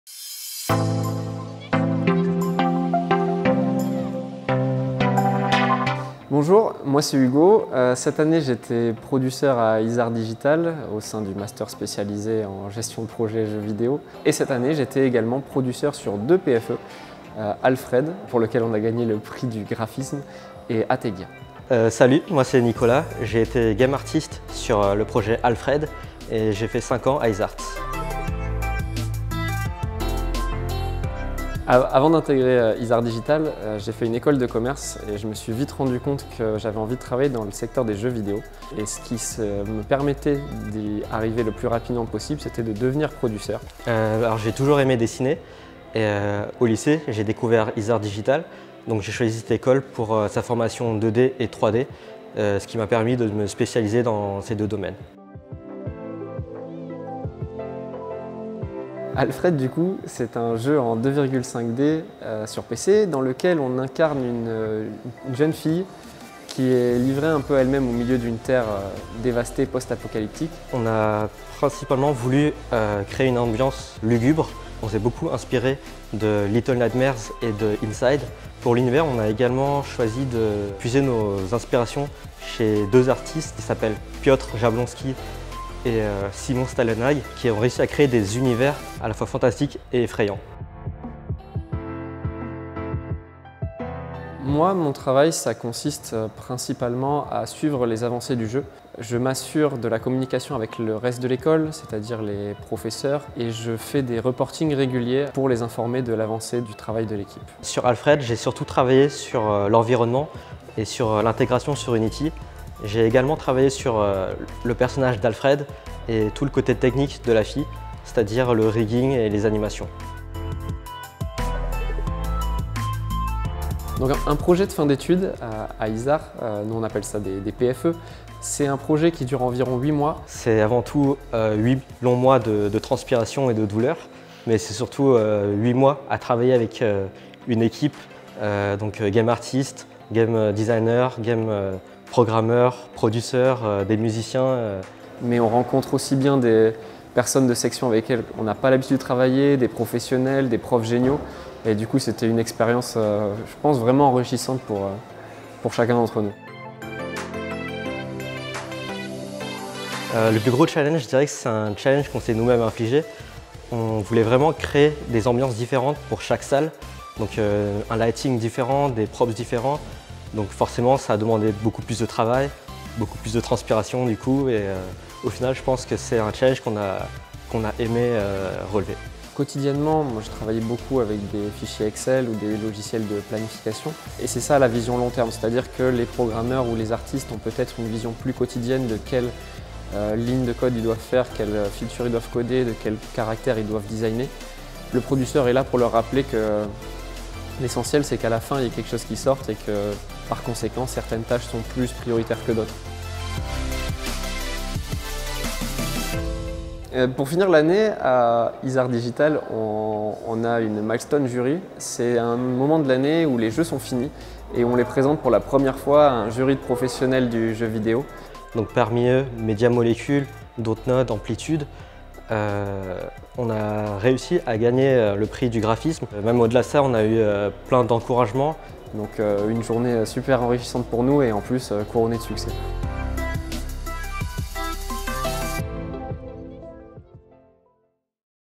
A.L.F.R.E.D. - INSIDE GAME ART & PRODUCER (2017)
GAME STORY : ALFRED

Interview de Hugo LEFEVRE et Nicolas THAM, Producer et Game Artist sur le jeu « A.L.F.R.E.D »

Incarnez une jeune fille perdue dans un monde dévasté, accompagnée d'A.L.F.R.E.D - un robot majordome qui obéit à une loi immuable : "les Robots doivent protéger les Humains". Vous serez donc en sécurité à ses côtés mais il se peut qu'il protège aussi vos adversaires humains.

Producer : Hugo LEFEVRE
Game Design : Joseph REICHENBACH, Romain JAOUHAR-GALLARD
Game Art : Nicolas THAM, Garance ROYERE LEBIGRE, Marion SPEISER
Game Design & Programming : Song HUANG
Game Programming : Delphine MIGEOT DE BARAN,
Music & Sound Design : Alexandre DE FRAMOND

Cadrage/Montage : Cyril VEYRET